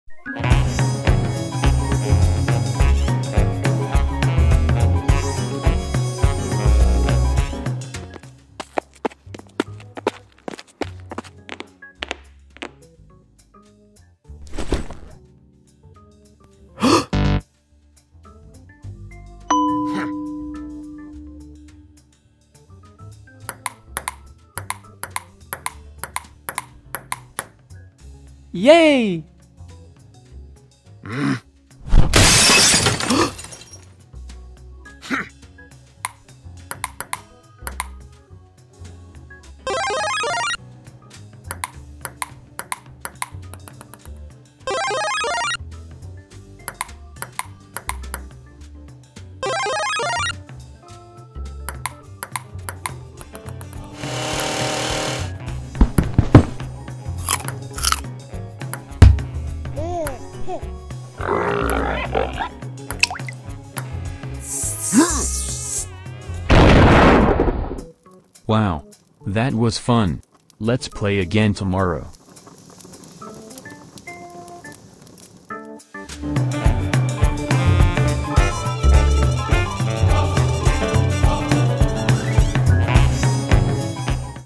Lego Ping Pong (Stop Motion)
You don't need a racket to play PING PONG. Watch this funny Lego Ping Pong Stop Motion to see why ;)